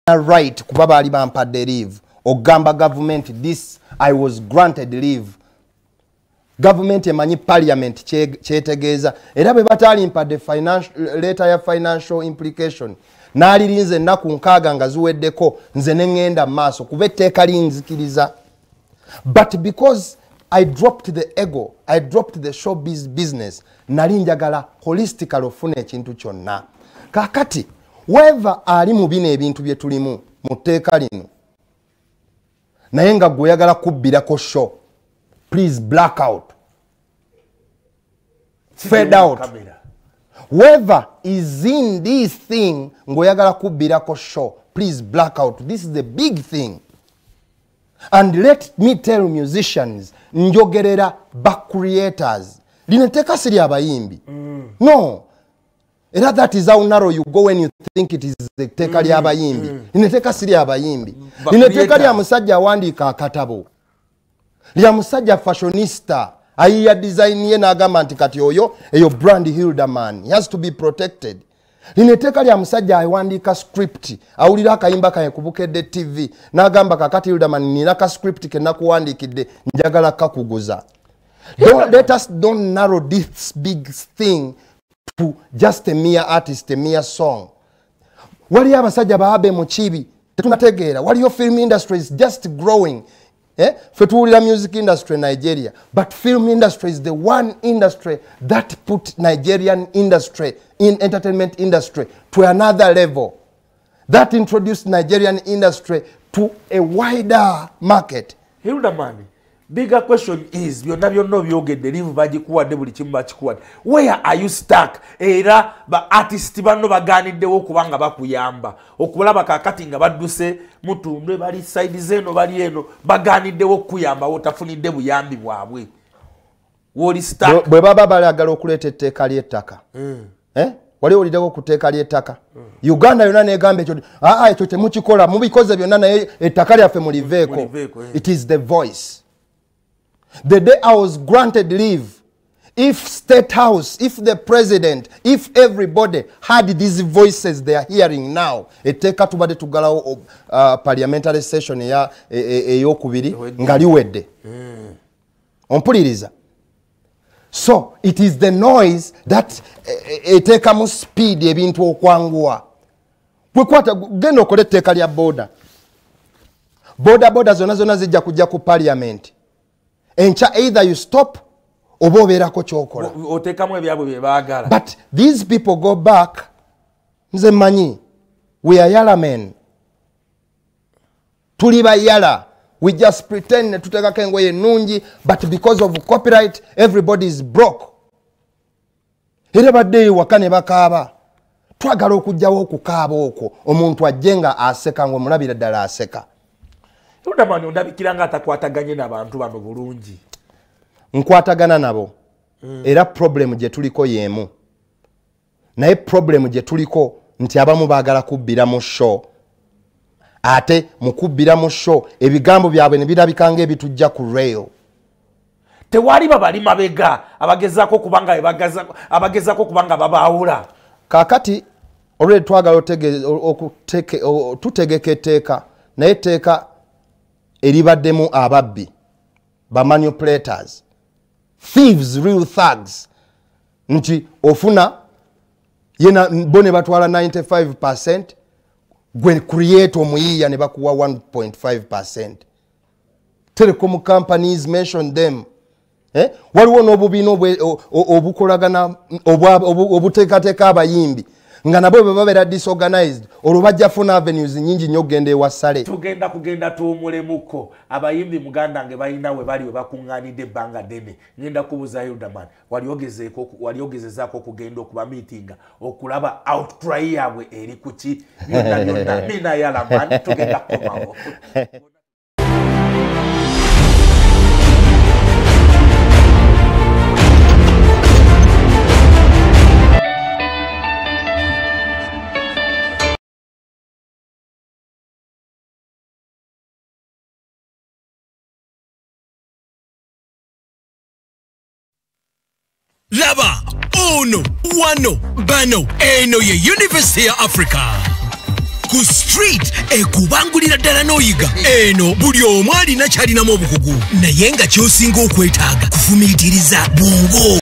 dr Hilderman to Musicians the copyright law is not yours alone do no be mistaken
Dr Hilderman askeredde abayimbi abagamba nti etteka lya copyright lya bayimbi bokka